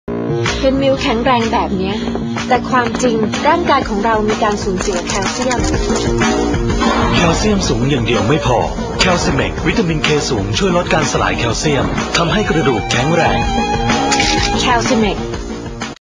โฆษณา นม Foremost Calcimex ปี 2551
พรีเซนเตอร์คือ หมิว ลลิตา

แอดLine:FOCUS 13
เบอร์โทรศัพท์:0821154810
และเป็นกำลังใจให้ช่องของเรา และผมจะลงต่อไปๆเรื่อย ด้วยนะครับ